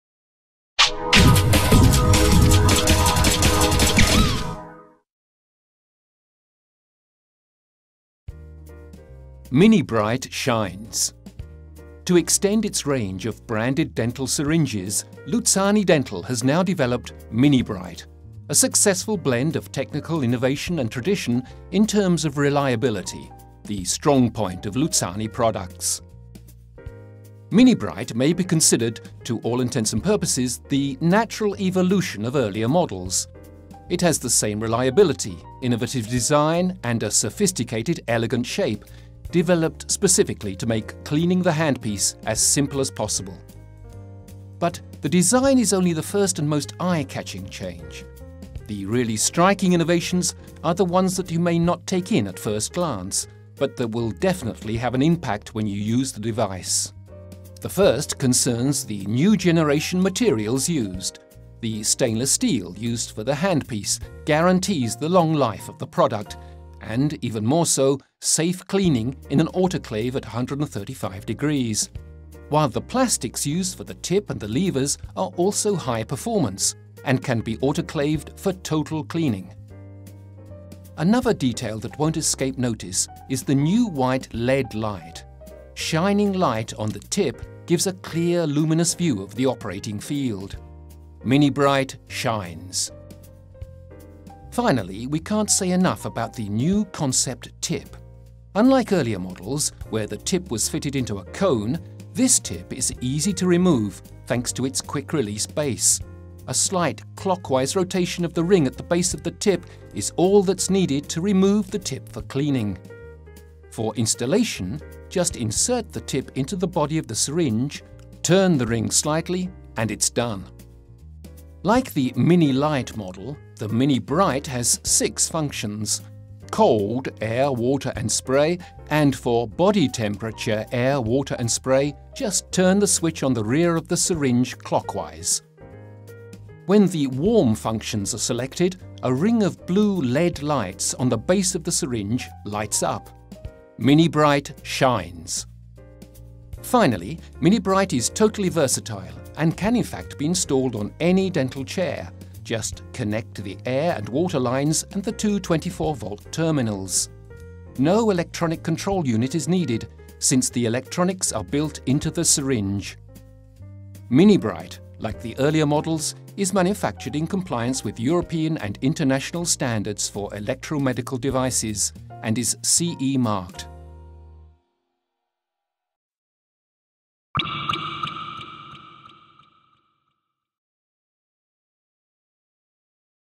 Luzzani presents MiniBright
MiniBright shines. To extend its range of branded dental syringes, Luzzani Dental has now developed MiniBright, a successful blend of technical innovation and tradition in terms of reliability, the strongpoint of Luzzani products.